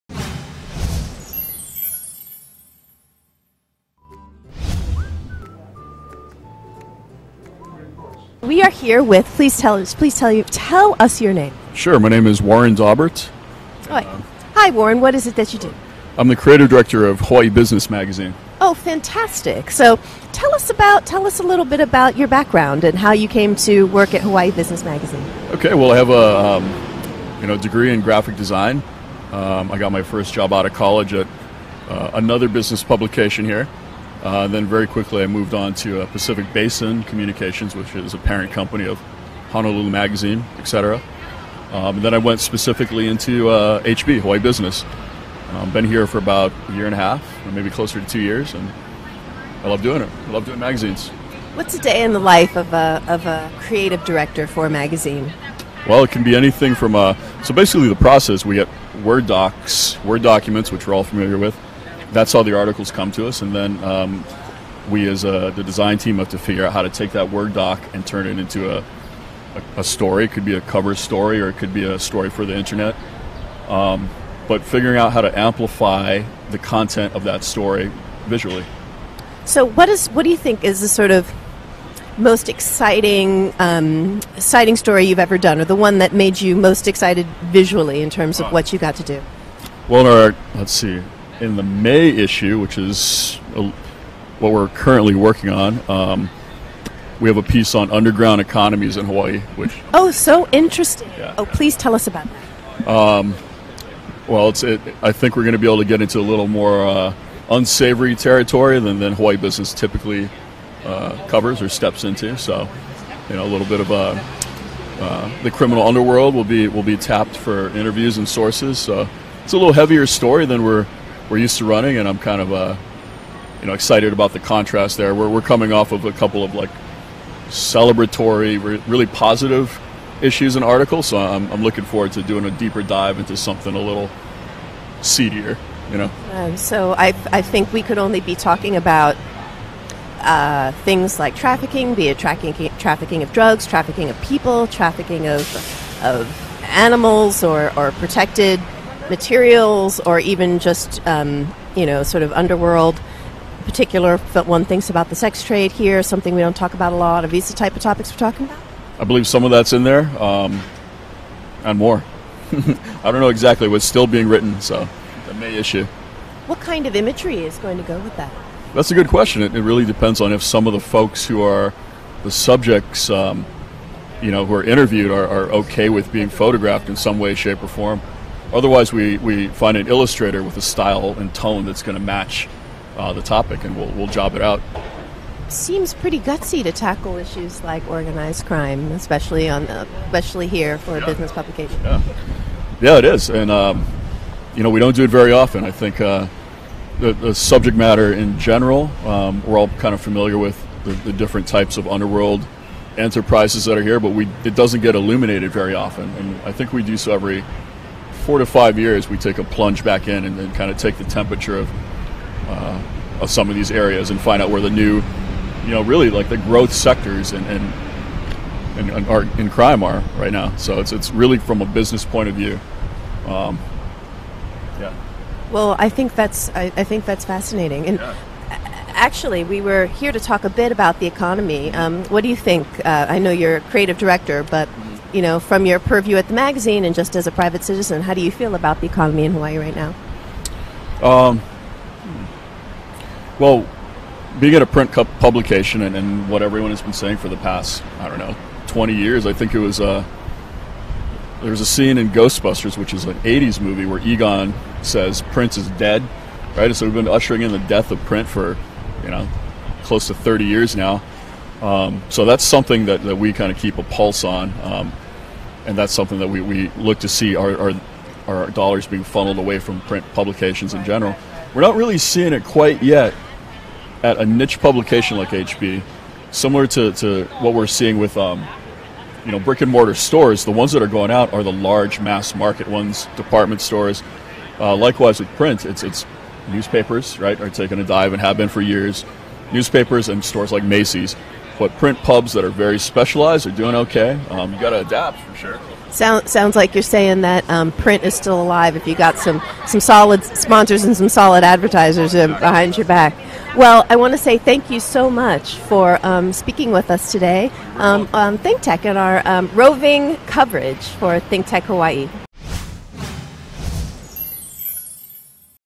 Tapping into the Underworld with Hawaii Business Magazine
Raya Salter visits Fort Street Mall to learn about the future of journalism in Hawaii with Warren Daubert, Creative Director of Hawaii Business Magazine.